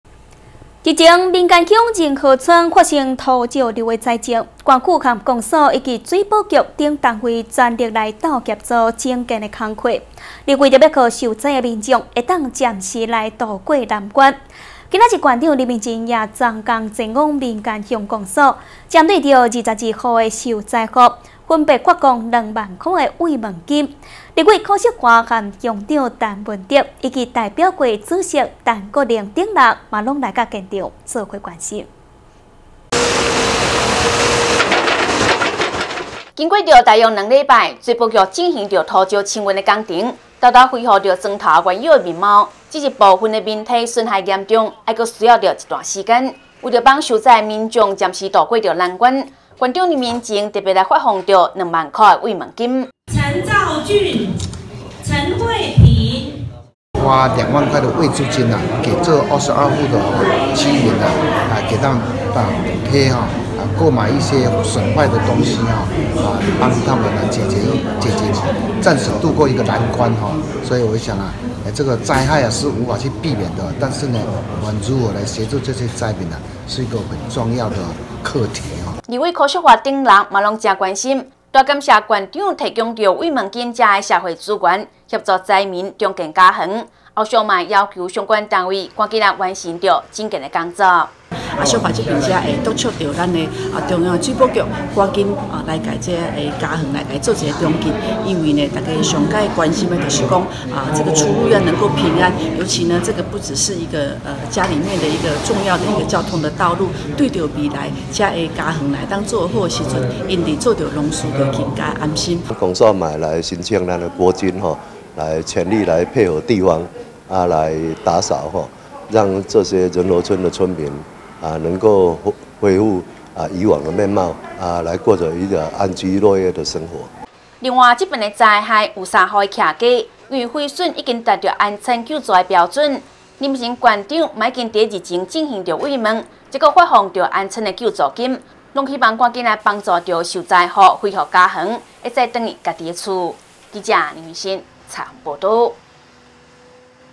南投新聞 名間致贈災民慰問金
日前名間鄉仁和村發生土石流災情，縣府、公所以及水保局等單位，全力協助整建工作，而為讓受災民眾暫時度過難關，今天縣長林明溱也專程前往名間鄉公所，針對22戶受災戶，分別發放2萬元慰問金，立委許淑華、鄉長陳文德、代表會主席陳國能等人也都到場一同關心。